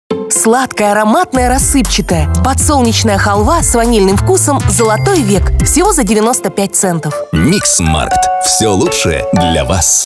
Halva // Top Angebote Mix Markt 24.02 – 29.02.20 // RU
Сладкая, ароматная рассыпчатая! Подсолнечная халва с ванильным вкусом "ЗОЛОТОЙ ВЕК" - всего за 95 центов!